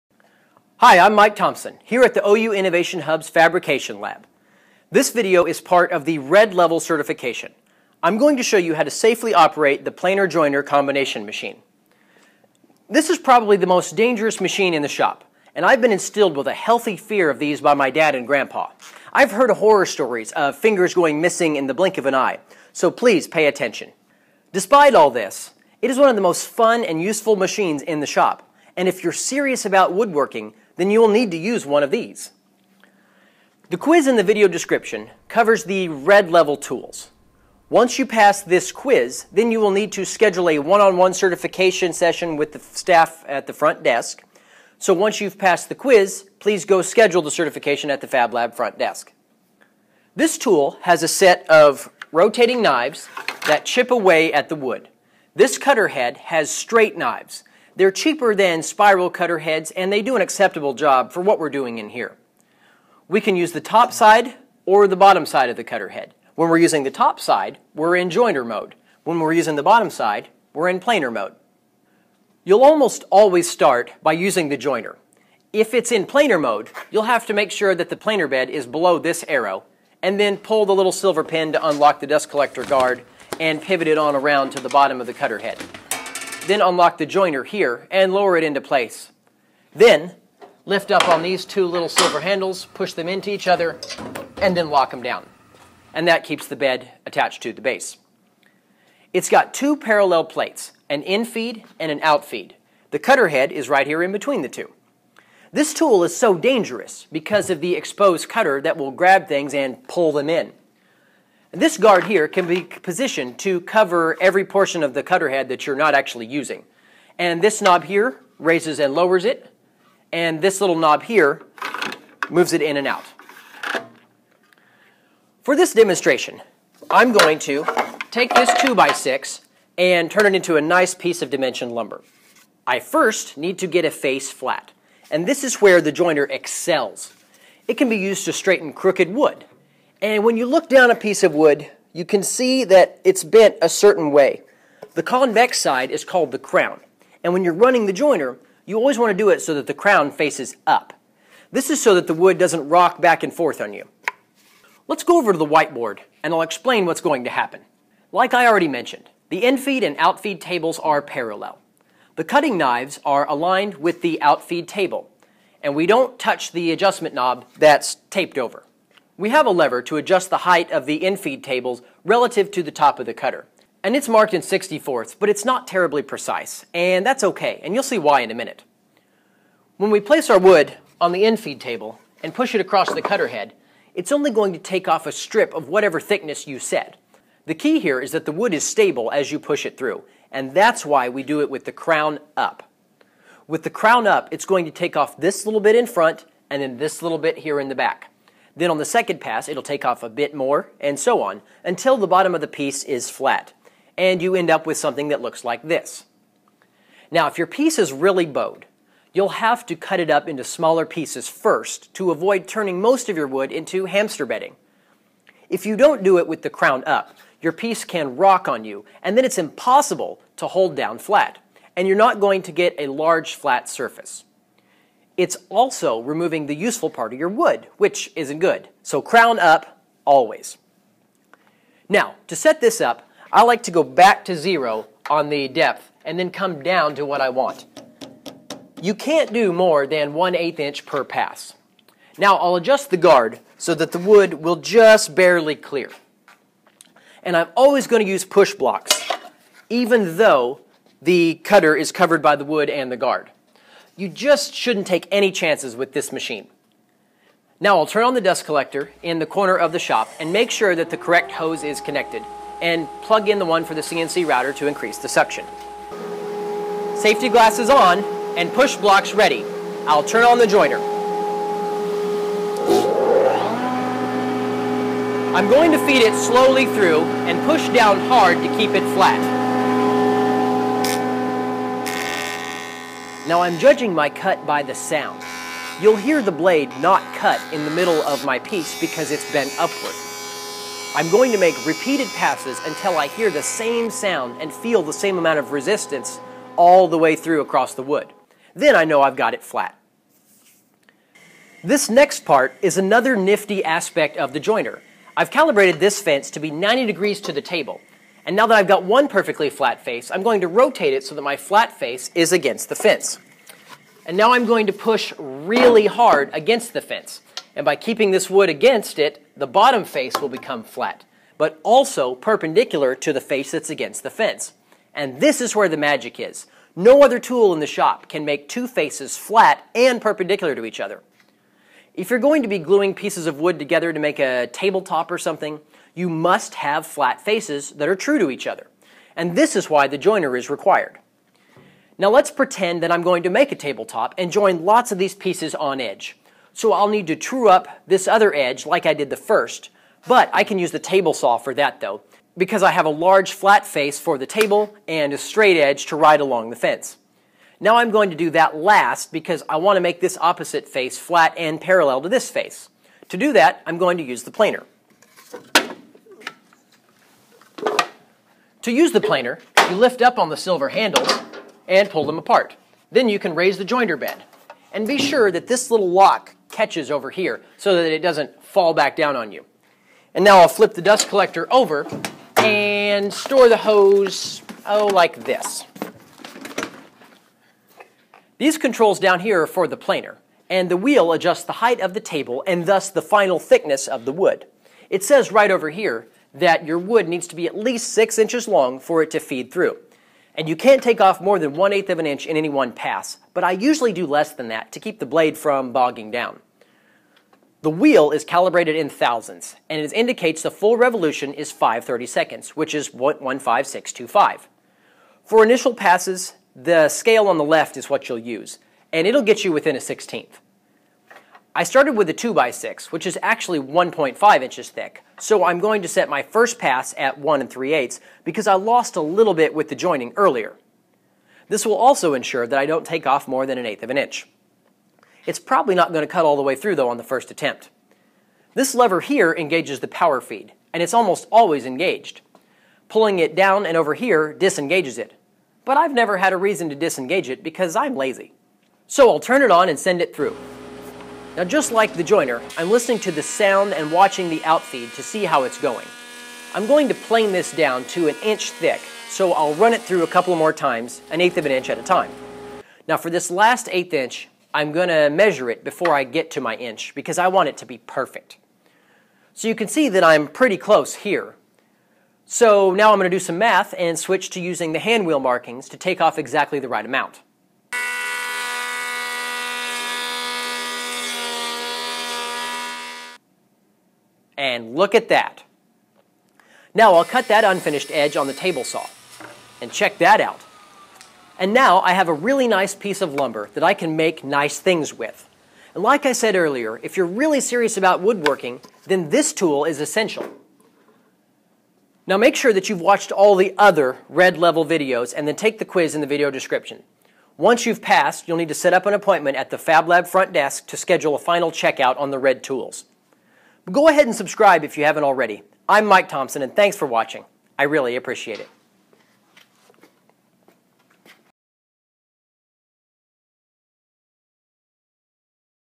iHub Safety Training - Jointer & Planer Certification
This video shows you how to safely operate our jointer planer machine. Then schedule a checkout session with the staff at the Fab Lab front desk.

Remember, you have to take our culture training before you can run any machines, even with certification:

Take the 3D Printer Certification Quiz now to gain access to the lasers: